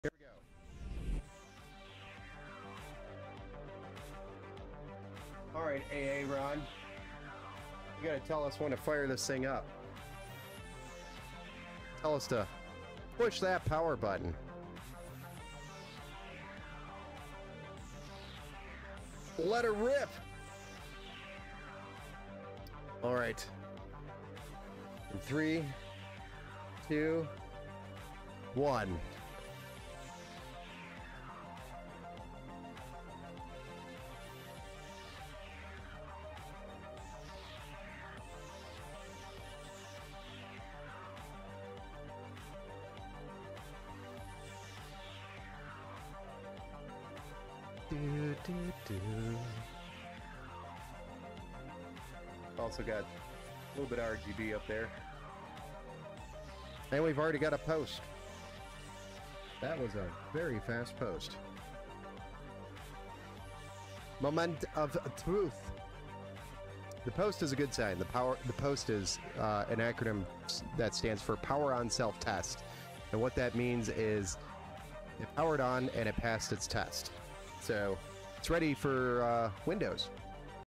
Is Your PC Alive? Decoding the Power-On Self-Test (POST)!
Join us as we power up a new system and achieve a successful POST! Witness the thrill of a fast POST and learn what it means for our computer. Get ready for Windows as we dive into this exciting build!